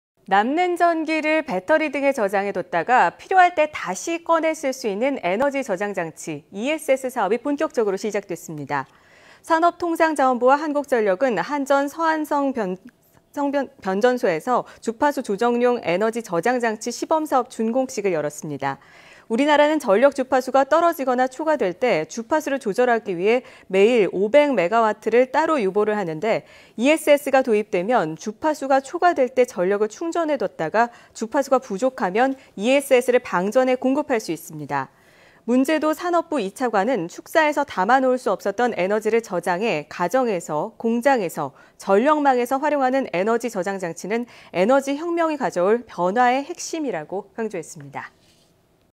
에너지 저장장치,ESS 국내 시장 열린다 / YTN 사이언스
남는 전기를 배터리 등에 저장해 뒀다가 필요할 때 다시 꺼내 쓸 수 있는 에너지 저장장치,ESS 사업이 본격적으로 시작됐습니다.

산업통상자원부와 한국전력은 한전 서안성변전소에서 주파수 조정용 에너지 저장장치 시범사업 준공식을 열었습니다.

우리나라는 전력 주파수가 떨어지거나 초과될 때 주파수를 조절하기 위해 매일 500MW를 따로 유보하는데, ESS가 도입되면 주파수가 초과될 때 전력을 충전해뒀다가 주파수가 부족하면 ESS를 방전해 공급할 수 있습니다.

문재도 산업부 2차관은 축사에서 담아 놓을 수 없었던 에너지를 저장해 가정에서, 공장에서, 전력망에서 활용하는 에너지 저장장치는 에너지 혁명이 가져올 변화의 핵심이라고 강조했습니다.